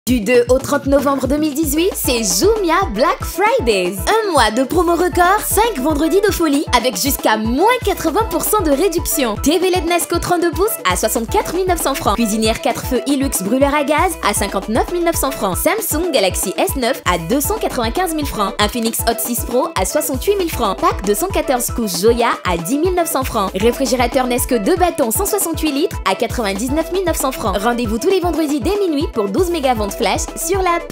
Jumia Black Friday spot
Du 02 au 30 Novembre 2018 JumiaBlackFridays ouvre ses portes ! 5 vendredis de folie, des réductions allant jusqu'à-80%, des chasses au trésor sur l'application mobile!